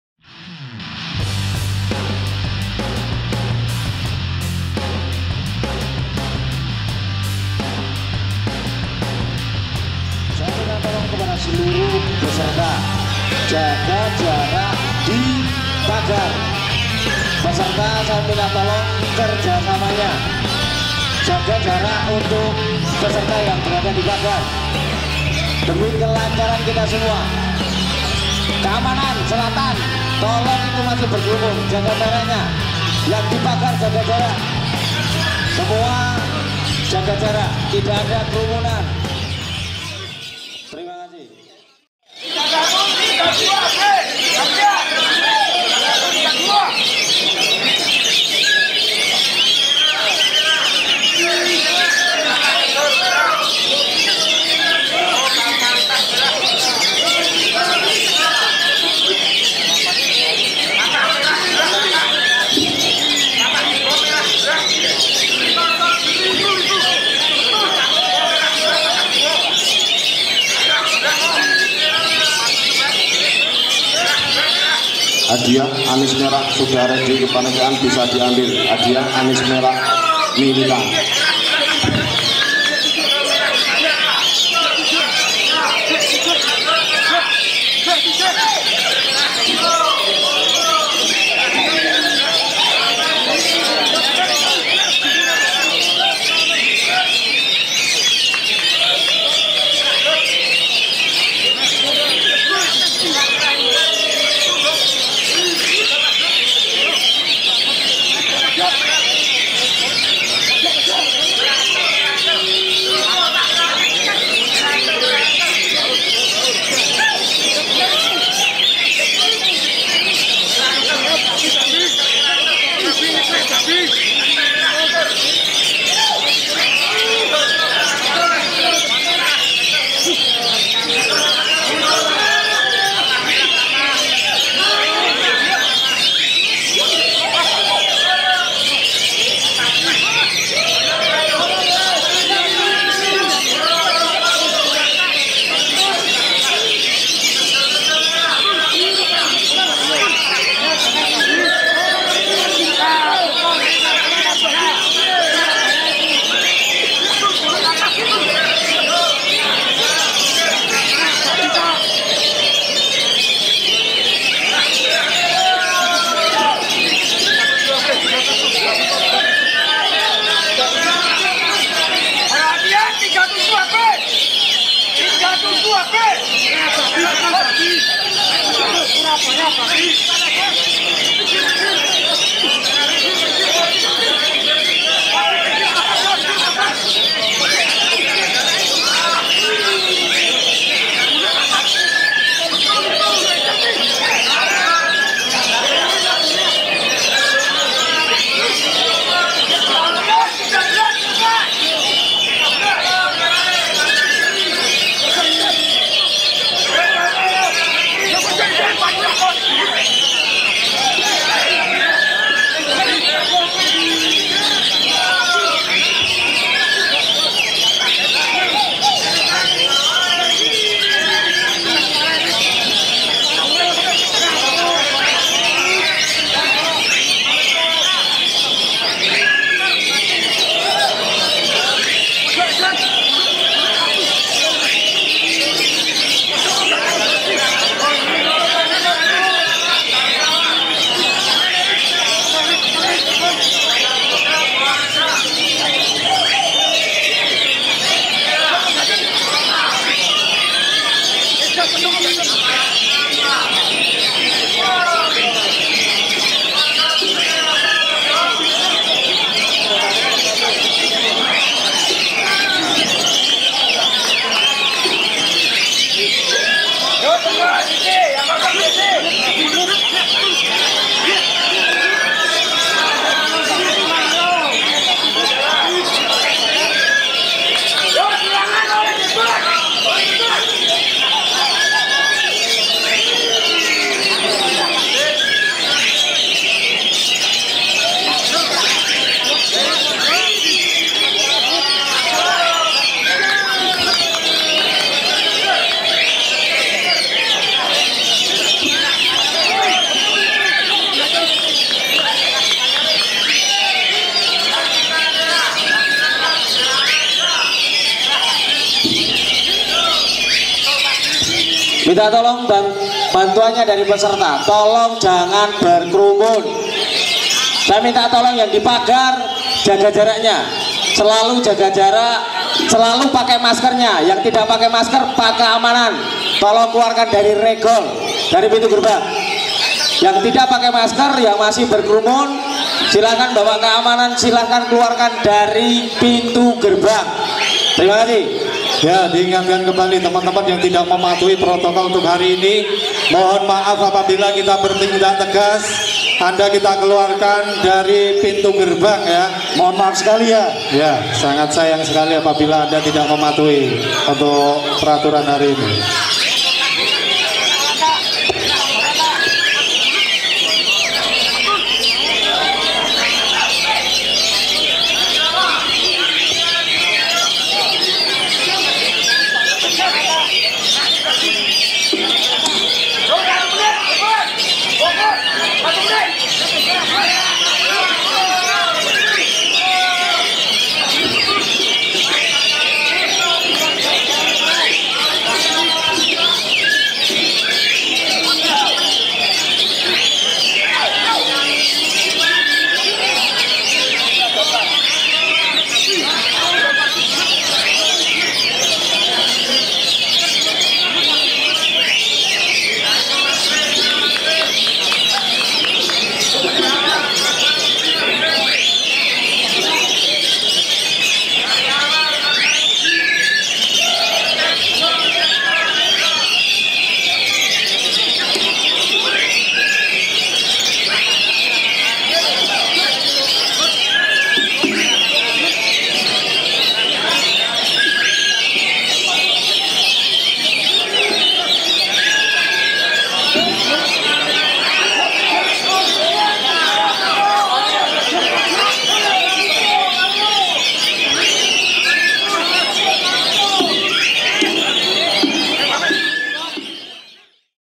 KACER JERUK PURUT - PANDORA BC WONOSOBO BERAKSI LAGI // KHI CUP IV YOGYAKARTA
liputanburung.web.id - Kacer kebanggan milik Mr. Tugio - Pandora BC Wonosobo kembali menampilkan aksinya. Berikut aksi Jeruk Purut saat berlaga digelaran even nasional KHI Cup IV Yogyakarta 7/2/2021